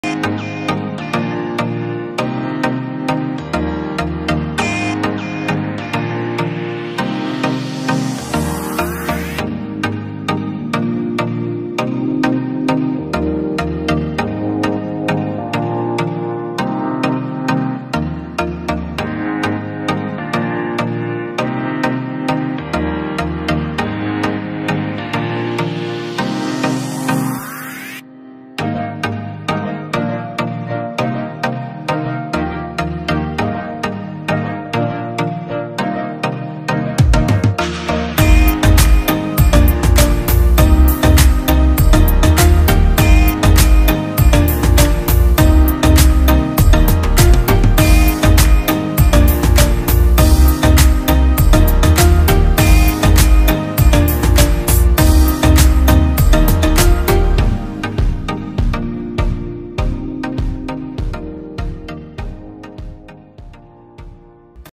Walking on ladders
Walking on ladders and sanding freshly installed crown moulding before painting.